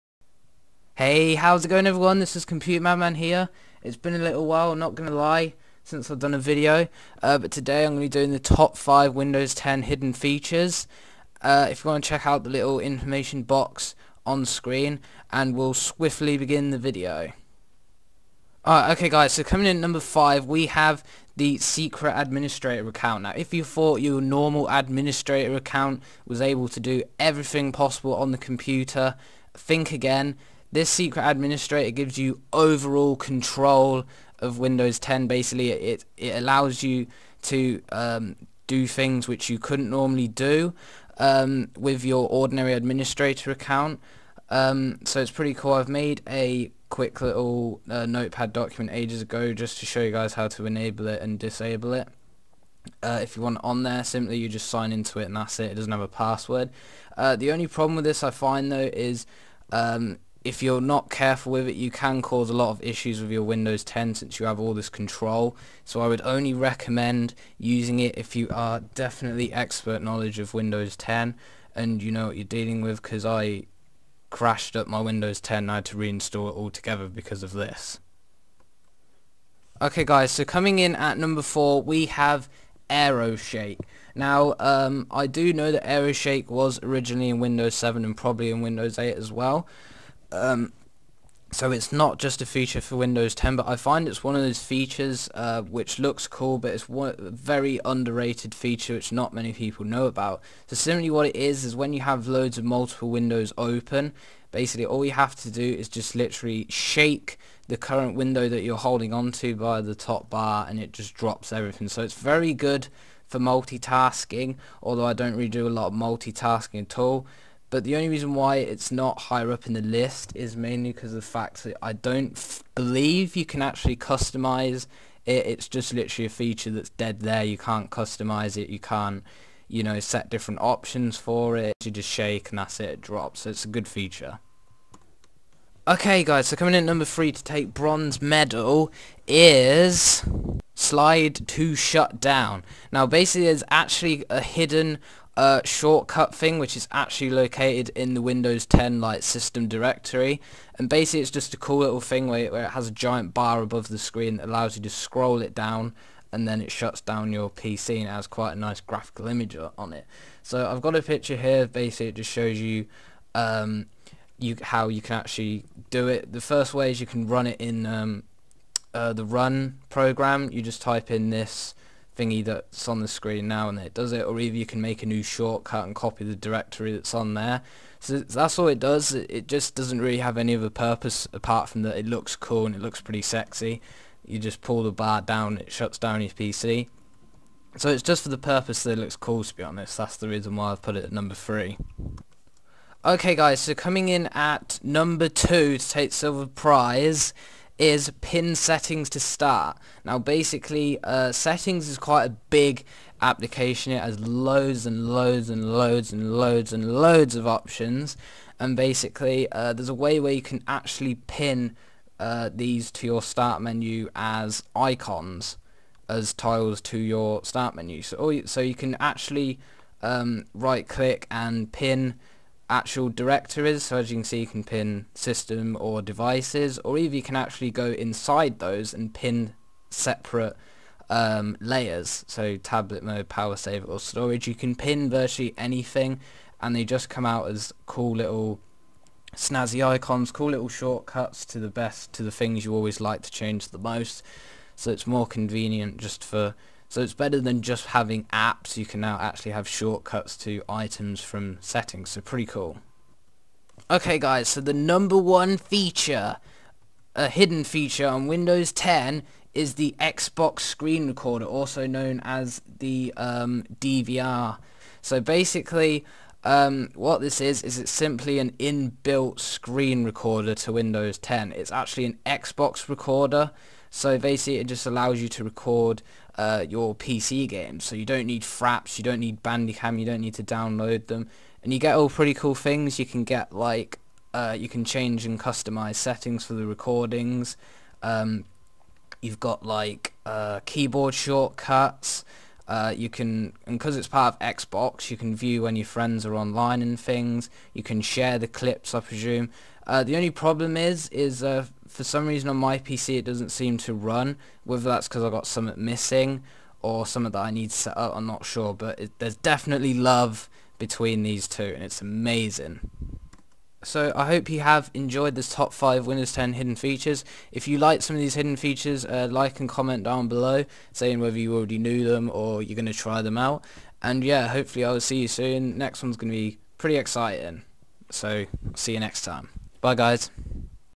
Top 5 Windows 10 Hidden Features
Hello everyone, want to have a go and try some Epicness hidden stuff found deep in Windows 10.
Sorry for long wait, but you should enjoy.

Interesting video lined up next, which I'm still currently planning. Should be ready soon hopefully.

Not much to say...
See you later. Bye ;)